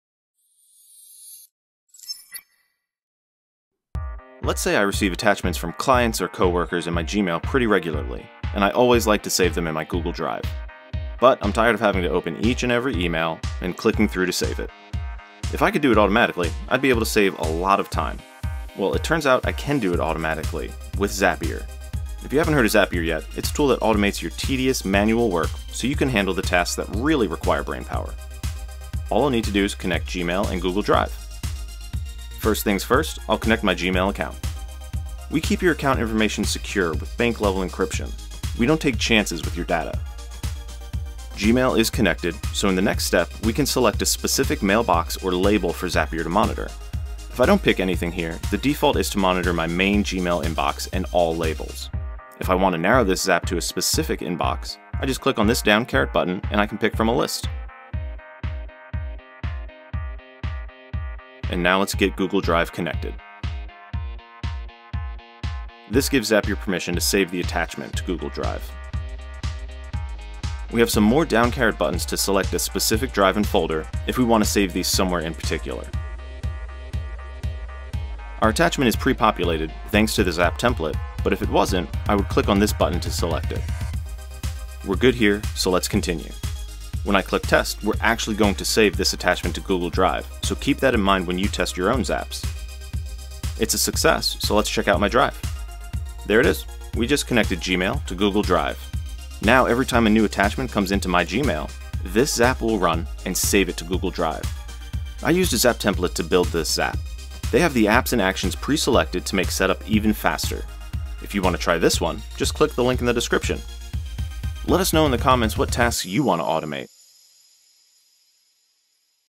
Updated: Connect Gmail to Google Drive - Create an Automatic Integration to Upload Attachments
What do you want to automate today?

Let's say I receive attachments from clients in my Gmail pretty regularly, and always save them to my Google Drive. But I'm tired of having to open each email and clicking through to save it.

If I could do it automatically, I'd be able to save a lot of time. Turns out, I can do it automatically With Zapier. If you haven’t heard of Zapier yet, it’s a tool that automates your tedious, manual work, so you can handle the tasks that really require brainpower.

All I’ll need to do is connect my Gmail and Google Drive accounts.

First things first, I’ll connect my Gmail account. We keep your account information secure with bank-level encryption—we don’t take chances with your data.

My Gmail account is connected. If we want, we can select a specific mailbox or label for Zapier to monitor. If I don't pick anything here, the default is to monitor the main Gmail inbox and all labels. If I want to narrow this Zap to a specific inbox, I just click on this down caret button and I can pick from a list.

Let's grab a sample email to use and we can get to connecting Google Drive.

This gives Zapier permission to save the attachment to Google Drive.

We have some more down caret buttons to select a specific drive and folder, if we want to save these somewhere in particular.

Our attachment is prepopulated, thanks to the Zap template, but if it wasn't, I would click on this button to select it.

We’re good here so let’s continue. When I click test, we’re actually going to save the attachment to Google Drive, so keep that in mind when you test your Zaps, too; Zapier is actually making the action happen.

It's a success, so let's check out my drive…and there it is. We just connected Gmail to Google Drive.

Now, every time a new attachment comes into Gmail, this Zap will run and save it to Google Drive.

I used a Zap template to build this Zap. They have the apps and actions pre-selected to make set-up even faster. What do you want to automate today?